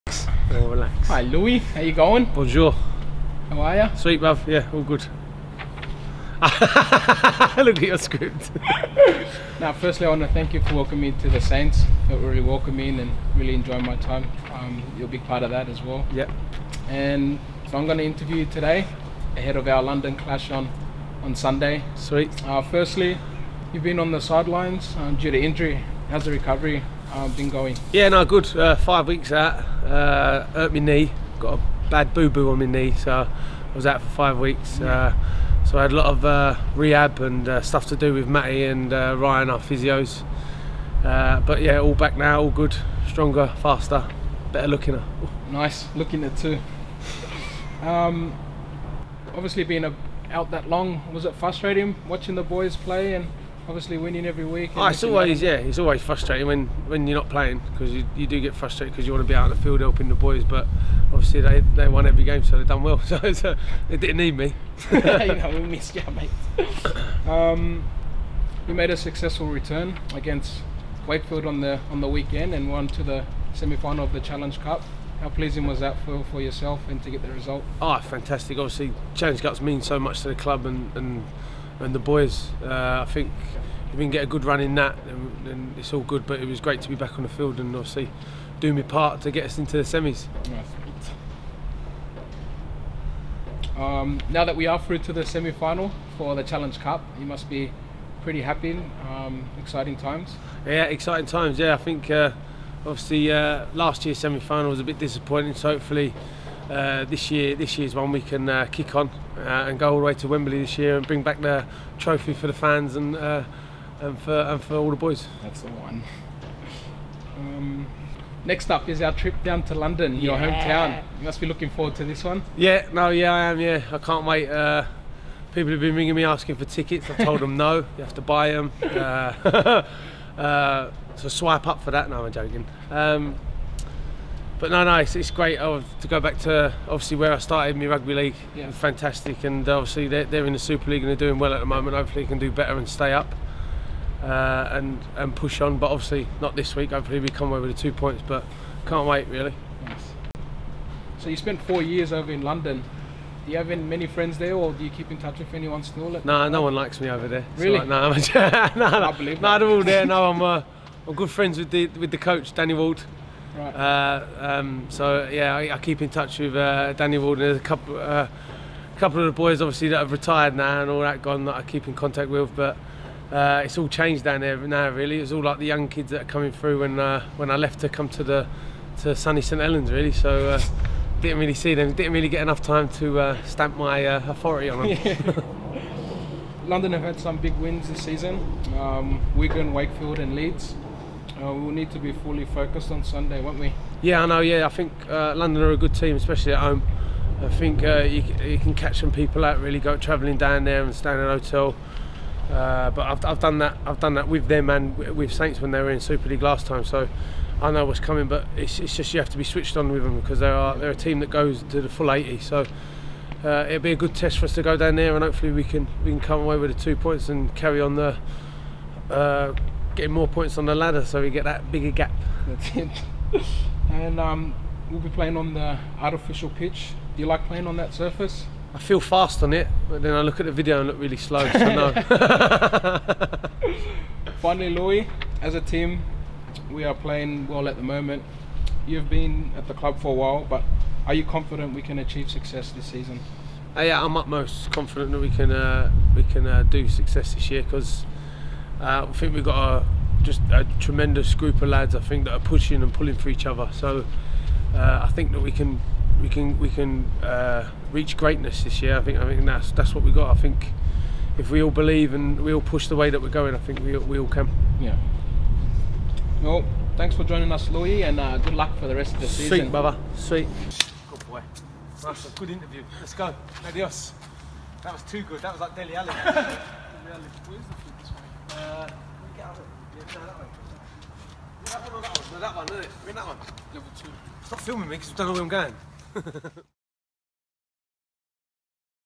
Kev interviews LMS ahead of his return to London!
Kev Naiqama took over media duties this week as he caught up with former London Broncos and passionate southerner, Louie McCarthy-Scarsbrook ahead his return home this Sunday. Wait for the personalised handshake at the end too.